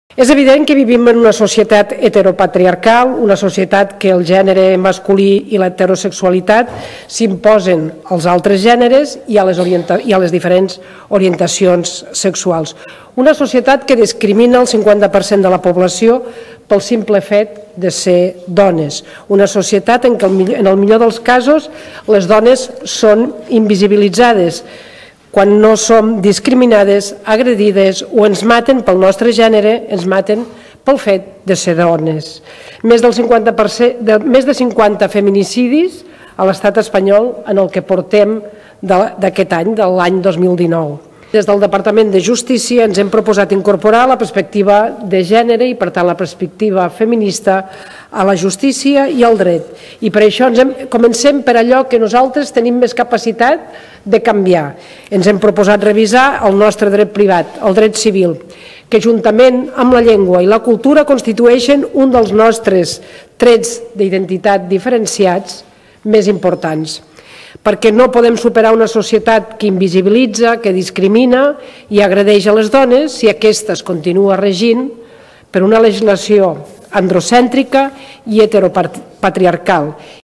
Revisió feminista de tot el Codi Civil de Catalunya. 23.07.19
La consellera Capella respon una interpelació al Parlament de Catalunya.